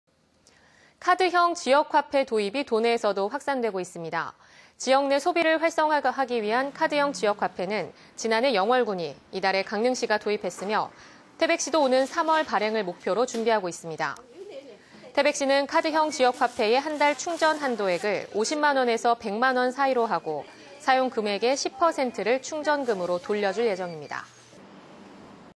[단신] 태백시, 카드형 지역화폐 3월 발행 추진 200122
카드형 지역화폐 도입이
도내에서도 확산되고 있습니다.
           ◀ＥＮＤ▶
 지역 내 소비를 활성화하기 위한
카드형 지역화폐는 지난해 영월군이,
이달에 강릉시가 도입했으며
태백시도 오는 3월 발행을 목표로
준비하고 있습니다.

 태백시는 카드형 지역화폐의
한 달 충전 한도액을
50만 원에서 100만 원 사이로 하고
사용 금액의 10%를
충전금으로 돌려줄 예정입니다. ///